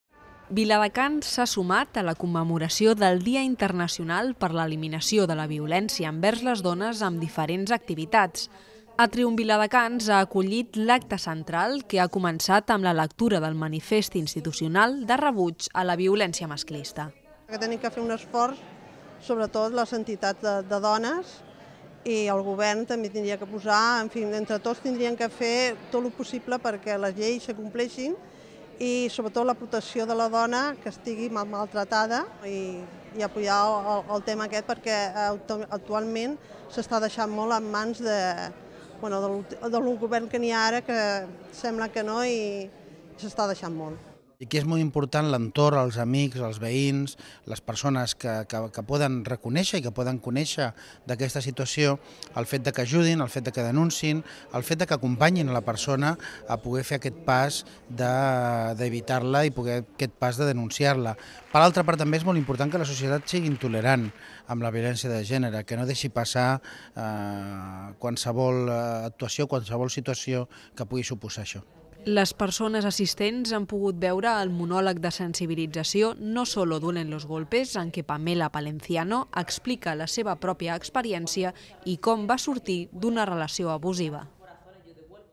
Viladecans commemora el Dia Internacional contra la violència masclista
Viladecans s’ha sumat a la commemoració del Dia Internacional per l’eliminació de la violència envers les dones amb diferents activitats. Atrium Viladecans ha acollit l’acte central que ha començat amb la lectura del manifest institucional de rebuig a la violència masclista.

Les persones assistents han pogut veure el monòleg de sensibilització ‘No sólo duelen los golpes’ en què Pamela Palenciano explica la seva pròpia experiència i com va sortir d’una relació abusiva.